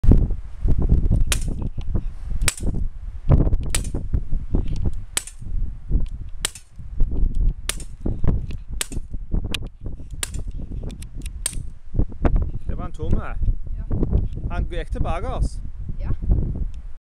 Hektor whit !New air gun that rekylere. Looks like a 22lr gun
New air gun that rekylere. Looks like a 22lr gun
Denne rekylerer og røyker!! Bedre kan det ikke bli